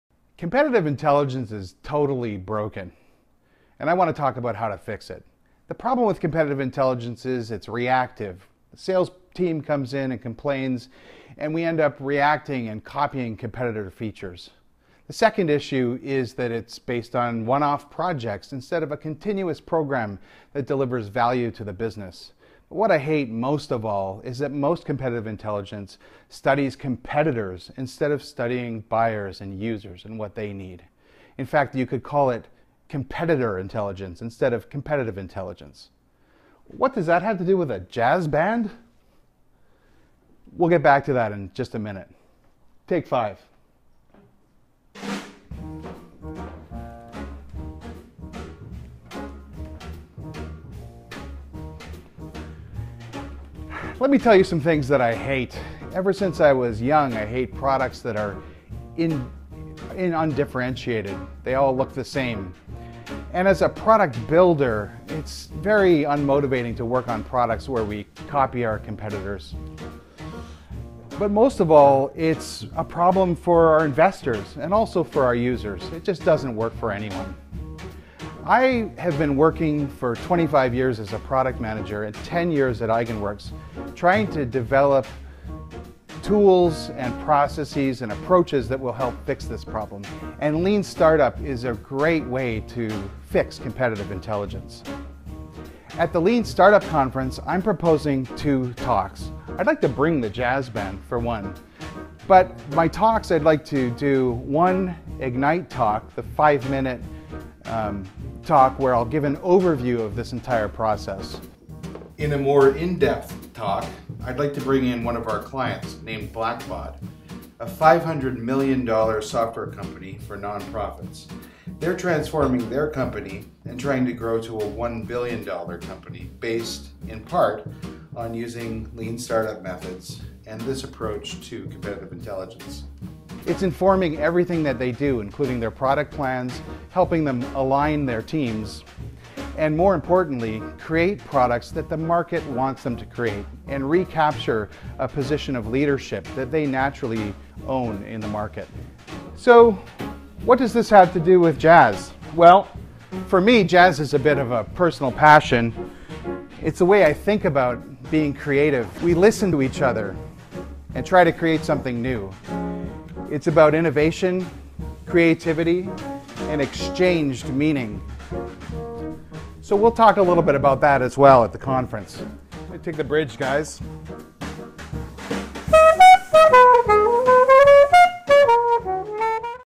Eigenworks Lean Startup Week 2017 Proposal Submission
Alan Armstrong makes his pitch for two talks he'd like to give this year at Lean Startup Week in San Francisco.
He fully intends to bring the jazz.